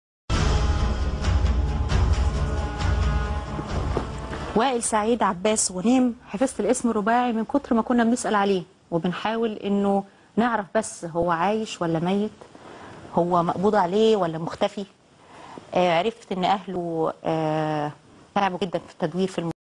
Who_Is_Ostaz_Chetos.mpeg
Is that on sceen logo for a cheesy-puff snack? DreamTV interview with Egyptian Google worker Wael Ghonim on his release from jail